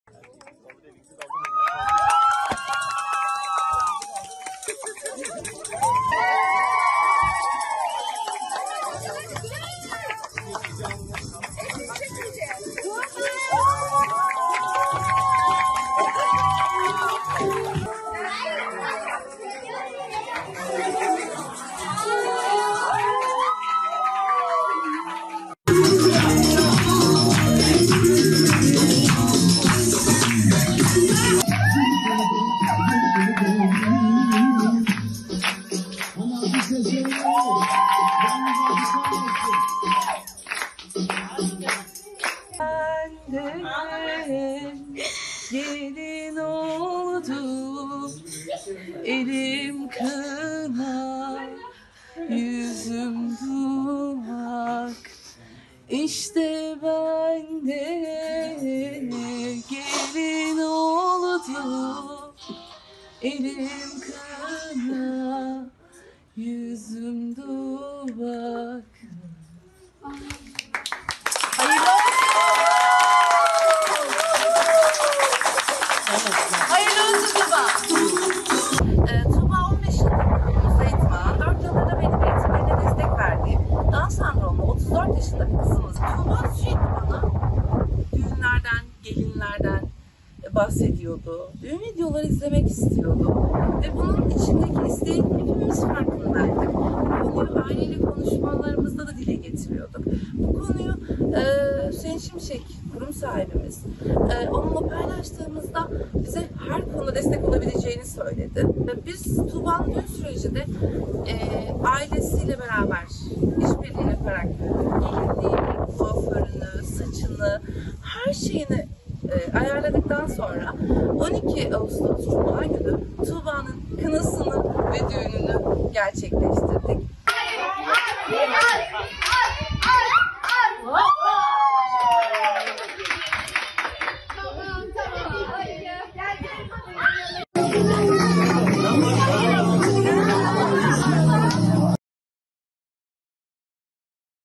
Down Sendromlu Tuğba'nın Düğün Hayali Gerçek Oldu!
Gaziantep'in Nurdağı İlçesinde, down sendromlu Tuğba Aksu'nun (34), beyaz gelinlik hayali, rehabilitasyon merkezindeki öğretmenler tarafından gerçekleştirildi. Gelinlik giyip, kınası yakılan Tuğba Aksu, sembolik düğünde doyasıya oynadı.